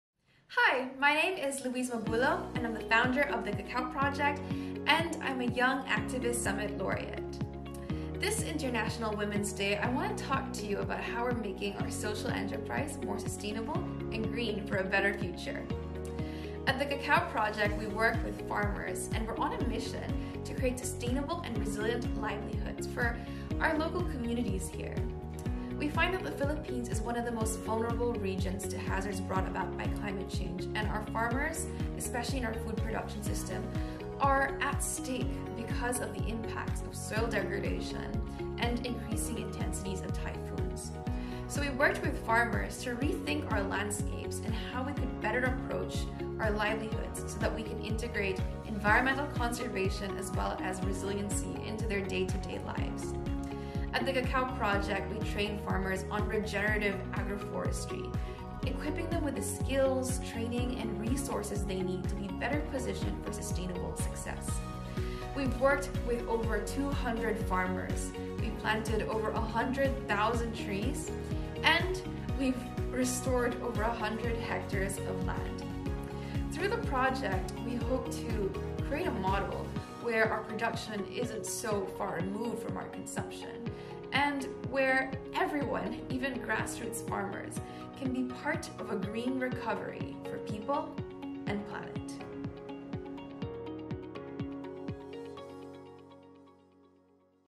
International Women's Day message from Louise Mabulo
💚She is going green - so can you!

We are celebrating IWD2022 by advocating gender equality in trade, and recognizing women's contribution in making trade more sustainable.

Meet Louise Mabulo, founder of the Cacao Project, an enterprise that cares about people & the planet🌏.